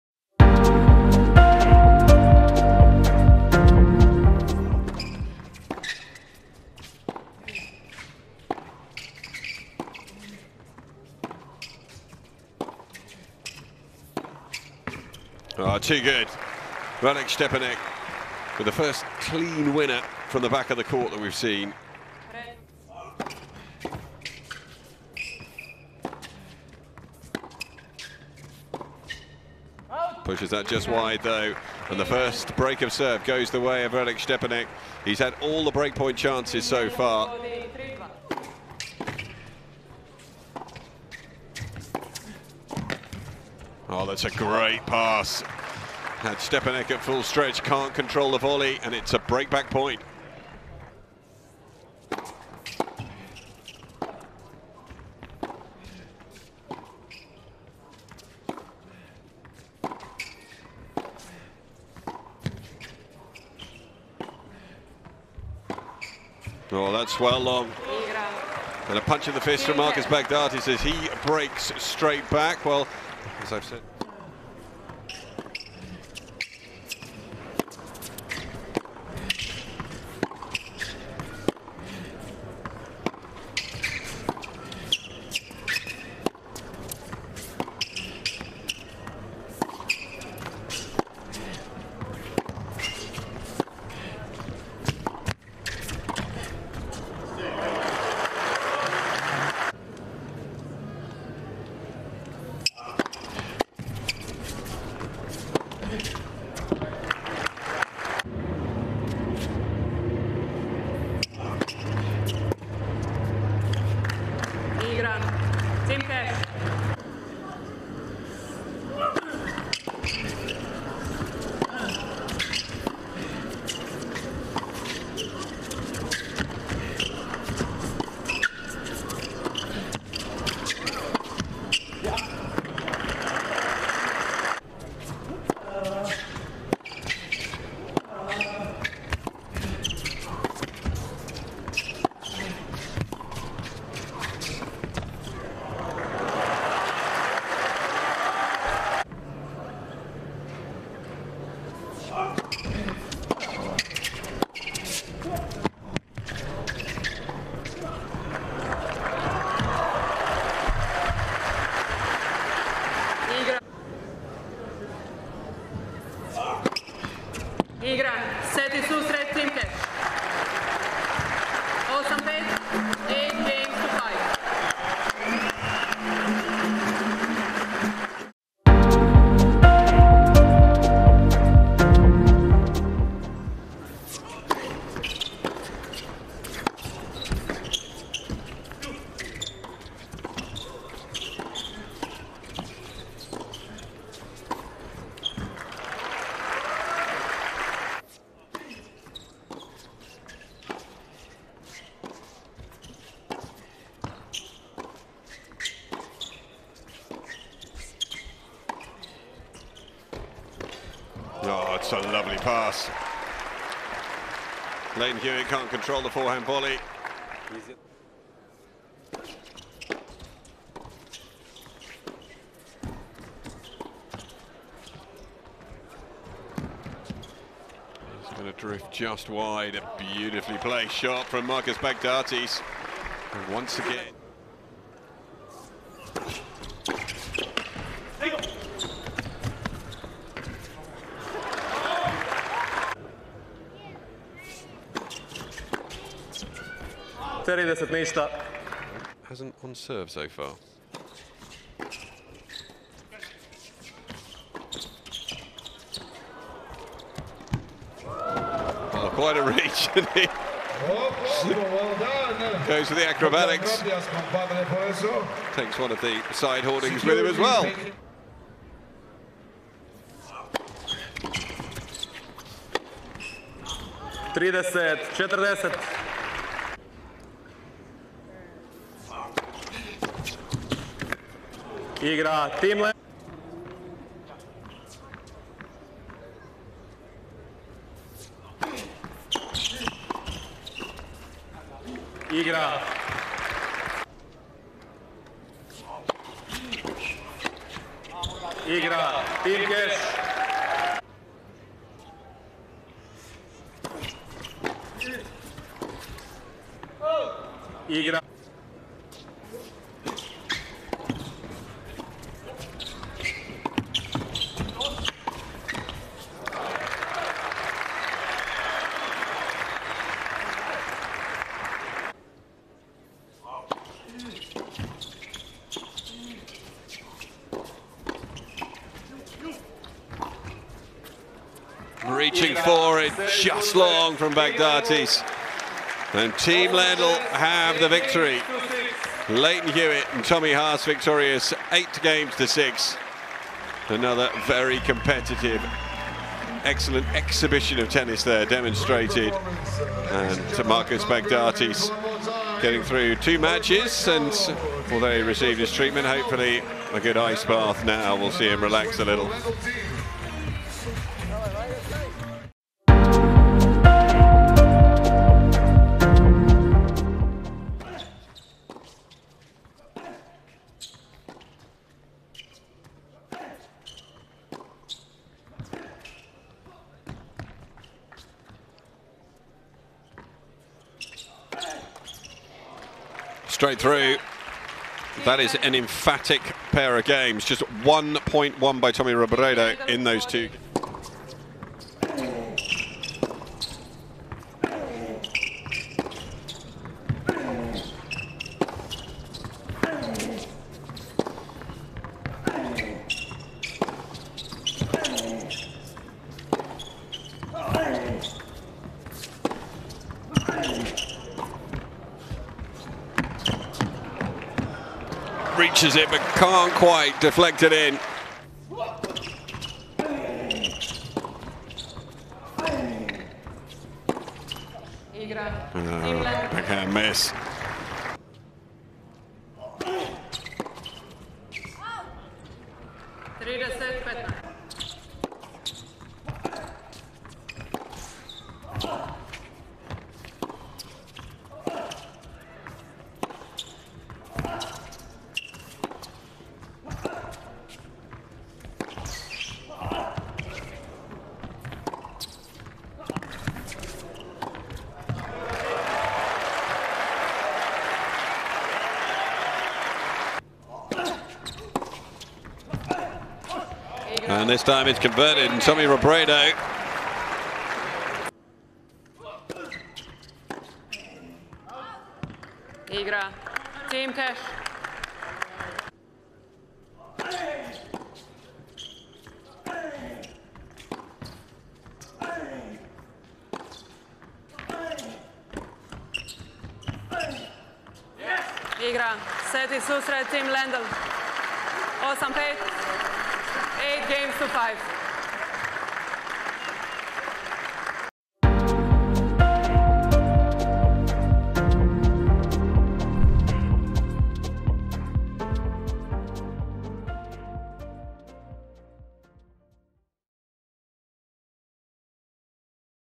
Ferrer vs Robredo; Nalbandian, Hewitt & Haas Feature | Legends Team Cup 2022 Day 1 Highlights
The first day of action from the inaugural Legends Team Cup event in Croatia.